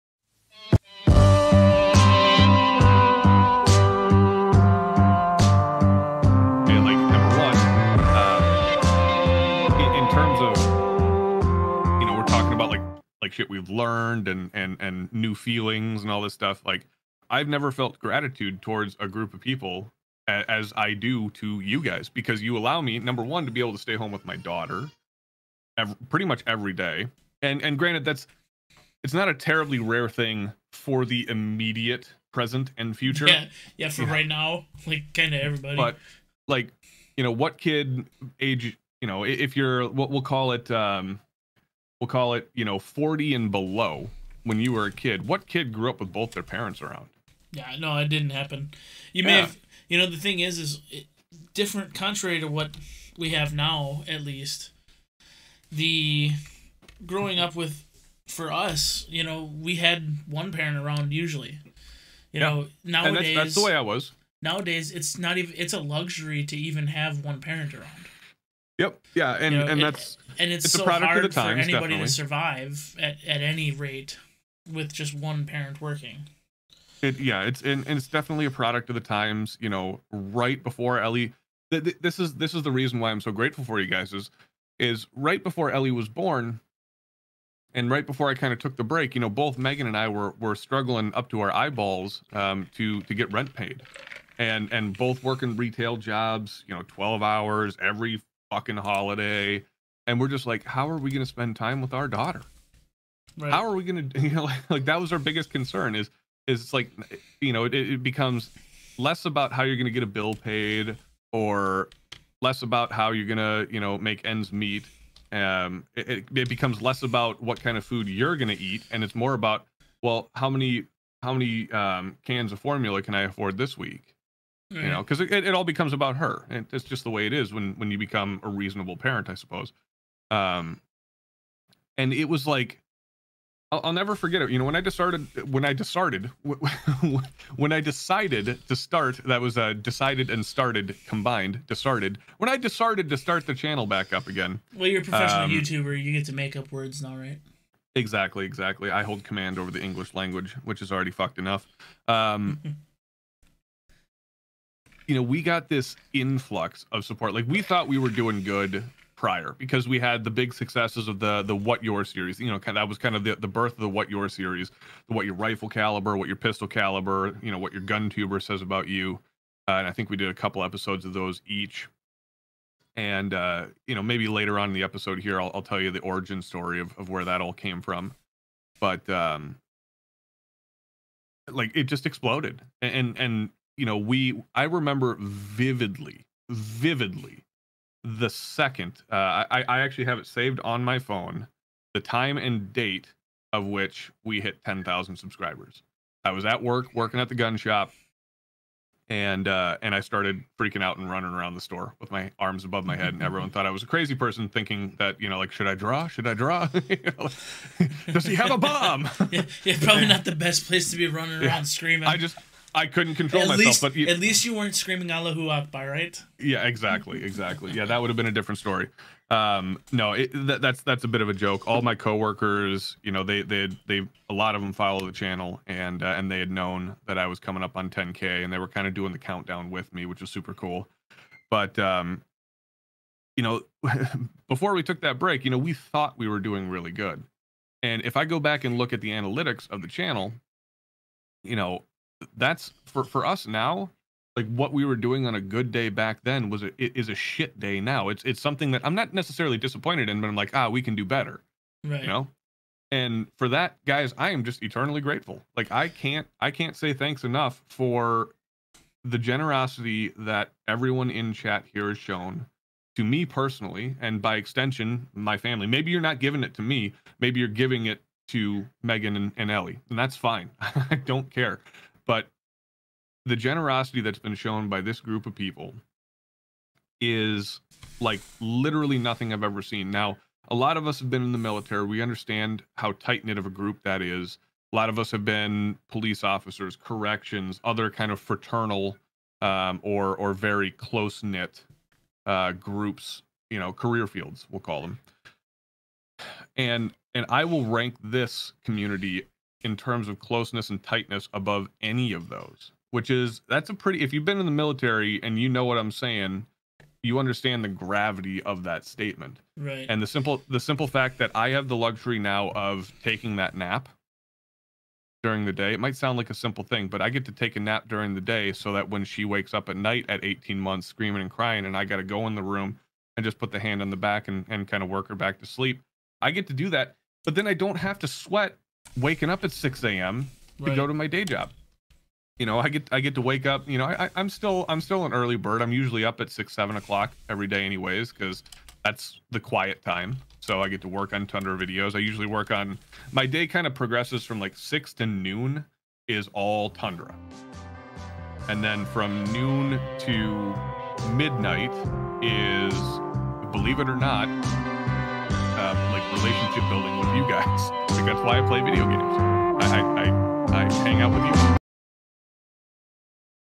Open Mic Clips: Tyler speaks to the Tundra Nation
Originally aired 08/21/2020 with guest Tyler from Tundra Tactical.  Bear with us as we get our clip chops worked out, but moving forward we will be releasing clip segments of the show to make viewing a little easier.

In this clip Tyler talks about his appreciation for the Tundra Nation.

If you are interested in hearing more of the conversation feel free to check out the entire conversation

for the most up to date listing of upcoming guests please